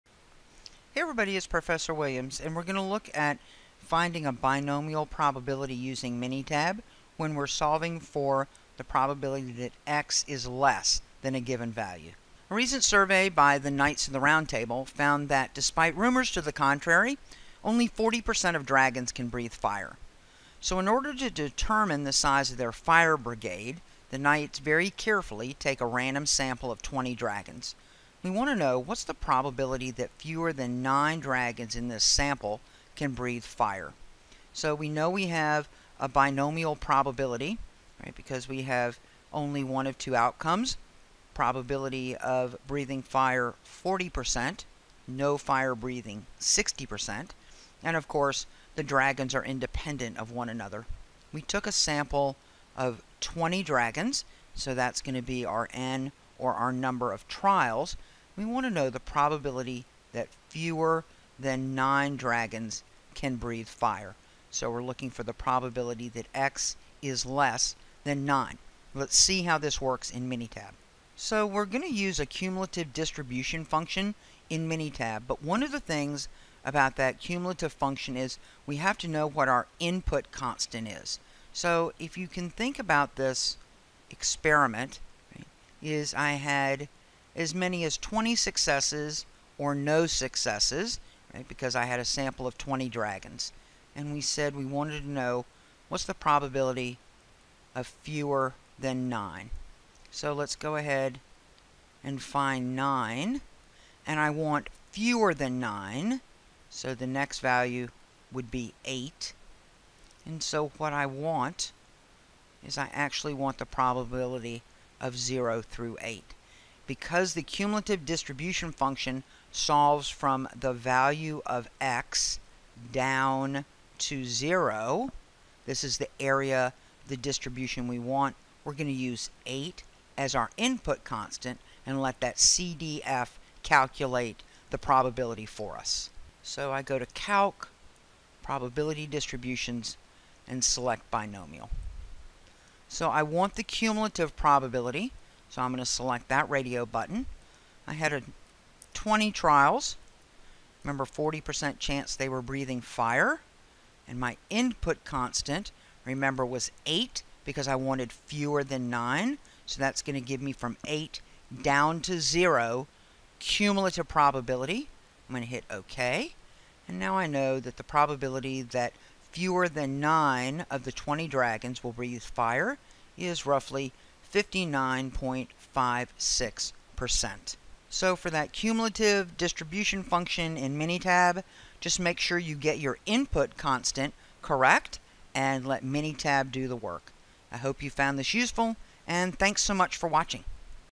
Statistics: Binomials on Minitab (Desktop) Solving for X less than X
This video demonstrates how to use the CDF function in Minitab to find the Binomial probability that X is less than a given value. In other words, what is the probability that we will have fewer than X successes out of n trials.